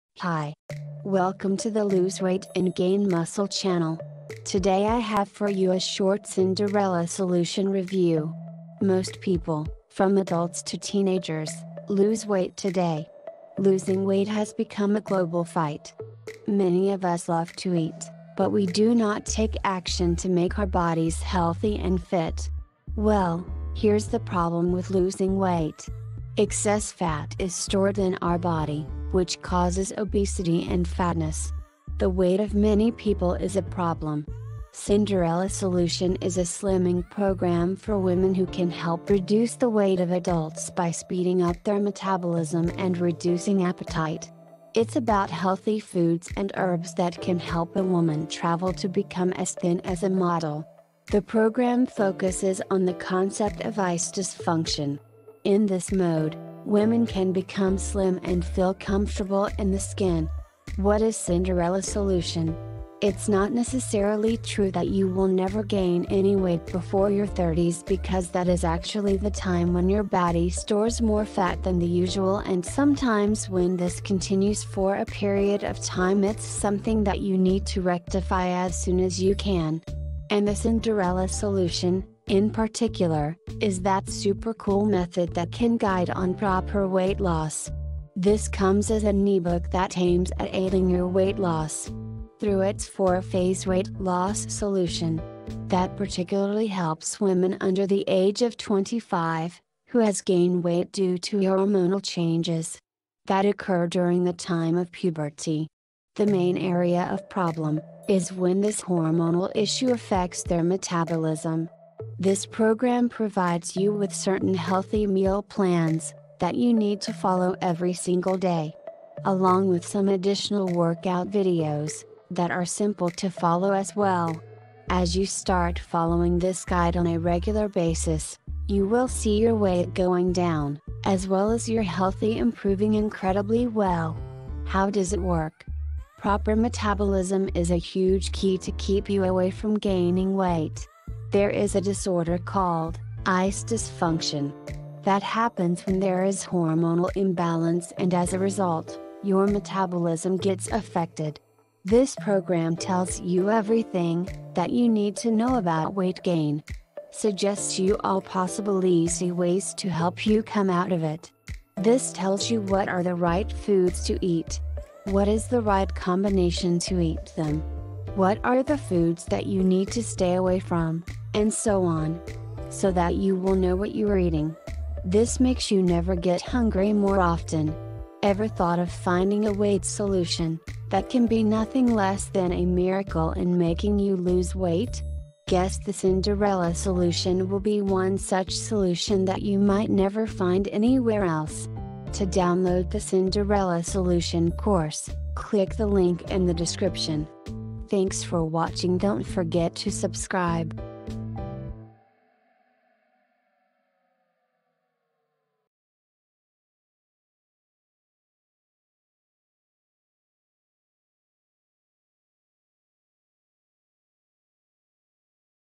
The Cinderella Solution Review | Is It Worth Your Money and Time?
Most people, from adults to teenagers, lose weight today. Losing weight has become a global fight. Many of us love to eat, but we do not take action to make our bodies healthy and fit. Well, here’s the problem with losing weight. Excess fat is stored in our body, which causes obesity and fatness. The weight of many people is a problem. Cinderella Solution is a slimming program for women who can help reduce the weight of adults by speeding up their metabolism and reducing appetite. It’s about healthy foods and herbs that can help a woman travel to become as thin as a model. The program focuses on the concept of ICE dysfunction. In this mode, women can become slim and feel comfortable in the skin. What is Cinderella Solution?It’s not necessarily true that you will never gain any weight before your 30’s because that is actually the time when your body stores more fat than the usual and sometimes when this continues for a period of time it’s something that you need to rectify as soon as you can. And the Cinderella Solution, in particular, is that super cool method that can guide on proper weight loss. This comes as an ebook that aims at aiding your weight loss. Through its four-phase weight loss solution. That particularly helps women under the age of 25, who has gained weight due to hormonal changes. That occur during the time of puberty. The main area of problem - is when this hormonal issue affects their metabolism. This program provides you with certain healthy meal plans, that you need to follow every single day. Along with some additional workout videos, that are simple to follow as well. As you start following this guide on a regular basis, you will see your weight going down, as well as your healthy improving incredibly well. How does it work? Proper metabolism is a huge key to keep you away from gaining weight. There is a disorder called - ICE Dysfunction. That happens when there is hormonal imbalance and as a result, your metabolism gets affected. This program tells you everything, that you need to know about weight gain. Suggests you all possible easy ways to help you come out of it. This tells you what are the right foods to eat. What is the right combination to eat them. What are the foods that you need to stay away from, and so on. So that you will know what you are eating. This makes you never get hungry more often. Ever thought of finding a weight solution, that can be nothing less than a miracle in making you lose weight? Guess the Cinderella Solution will be one such solution that you might never find anywhere else.